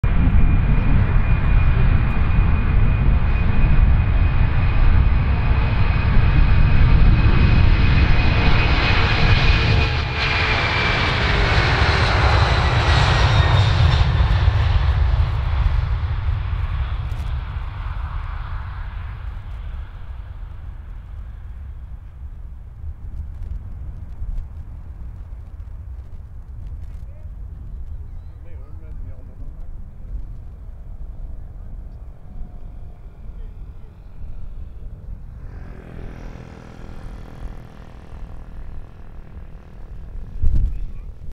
KLM B747-400 Takeoff from Schiphol
Airline: KLM
Aircraft: Boeing 747-406
Flight: KL611
Route: AMS-ORD
Date: 7th of April, 2019